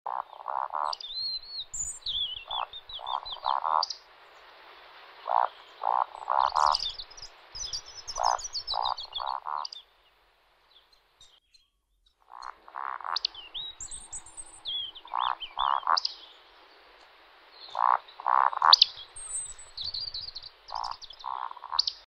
"Roding" Courtship flight in spring - Eurasian Woodcock
The male of Eurasian Woodcock performs a courtship flight known as "roding" at dusk in spring. It is widely believed that the female will sometimes carry chicks between her legs whilst flying, though evidence of this is purely anecdotal. The world population is estimated to be 14 million to 16 million birds.